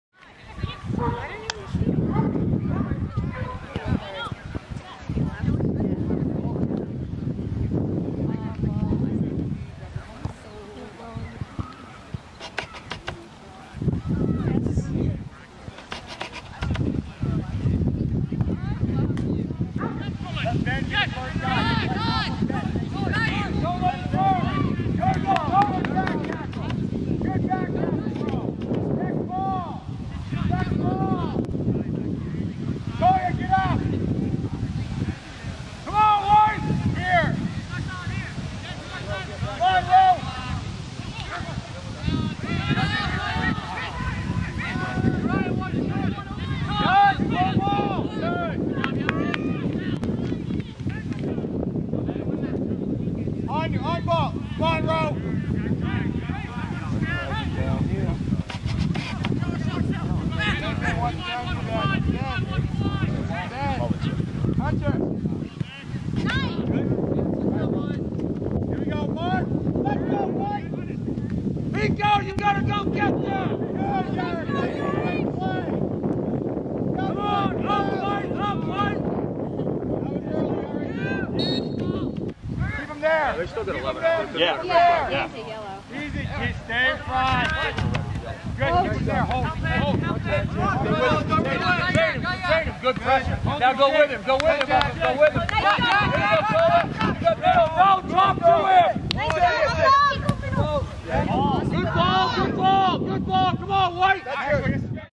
Lincoln Academy vs Oceanside boys soccer
Scenes from Lincoln Academy versus Oceanside high school boys varsity soccer on Oct. 8 in Thomaston.